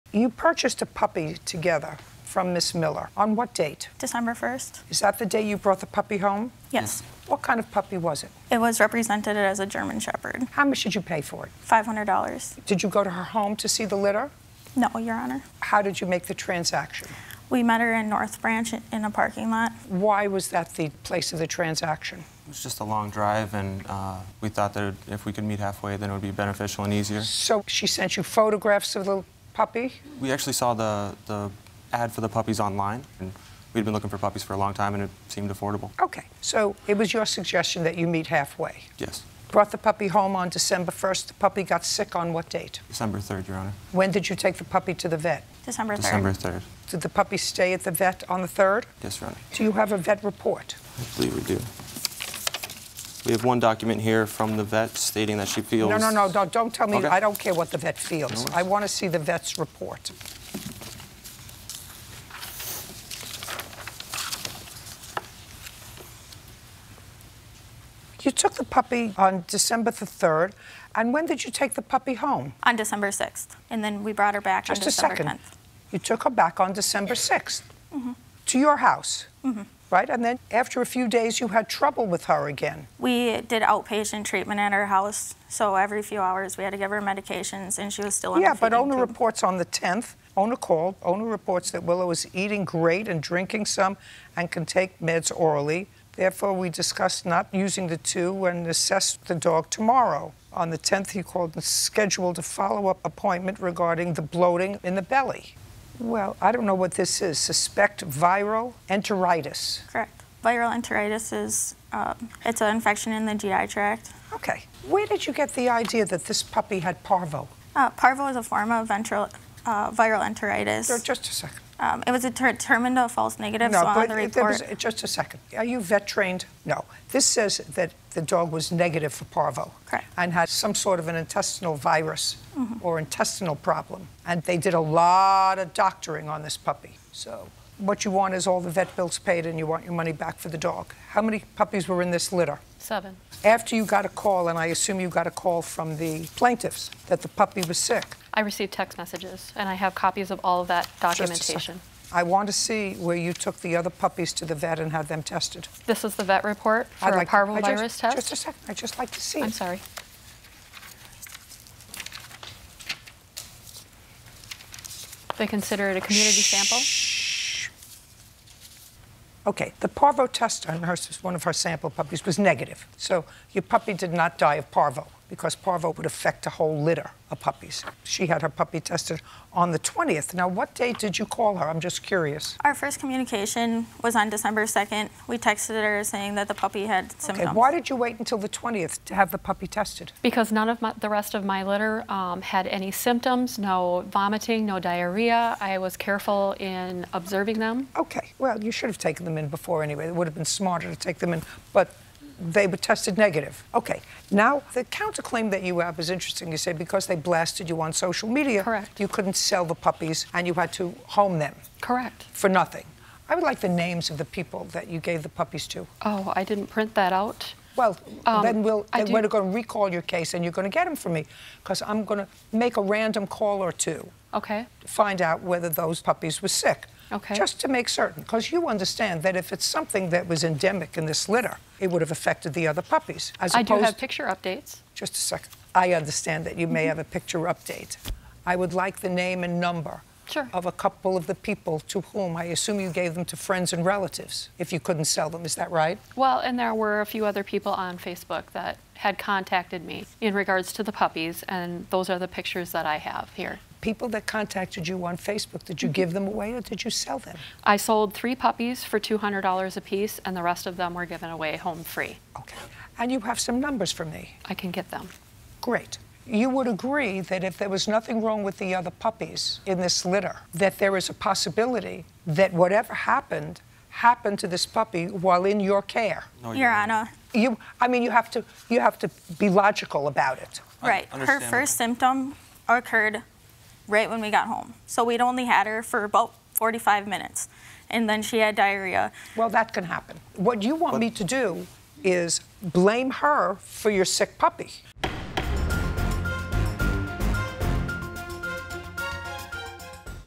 Did Woman Knowingly Sell a Sick Puppy? | Part 1
Scott and Mariah bought a German Shepherd puppy from Tonja -- and it got sick and died!